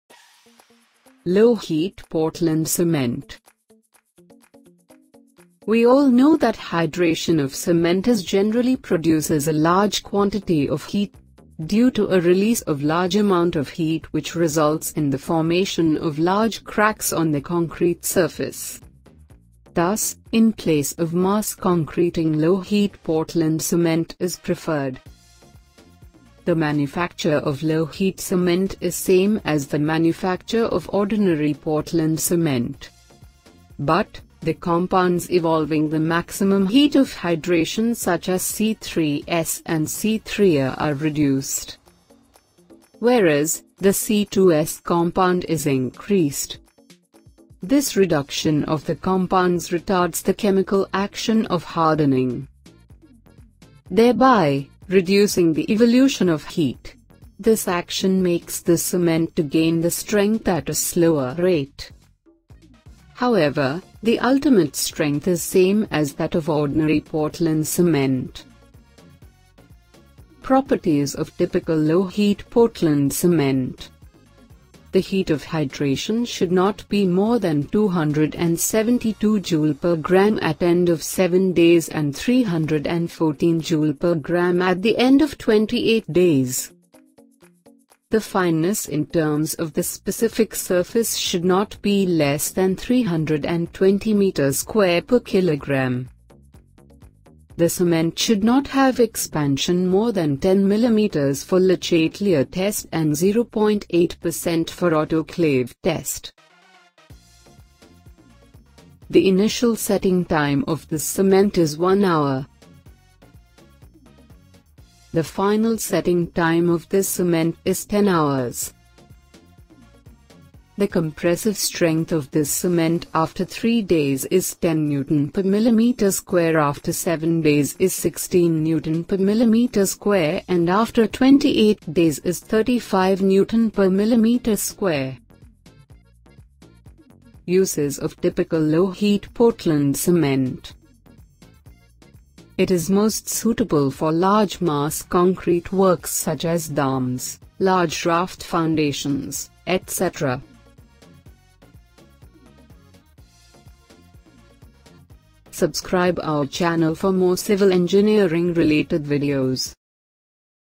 What is Low Heat Portland Cement? || Properties ||Uses || Types of Cement #5
In this informative video, we dive deep into the world of Low Heat Portland Cement, exploring its fascinating properties, diverse uses, and various types. Join us as we uncover the secrets behind this specialized cement that minimizes heat generation during hydration. From construction projects to infrastructure development, LowHeatPortlandCement is revolutionizing the industry. Expand your knowledge and stay ahead of the game with Cement 101!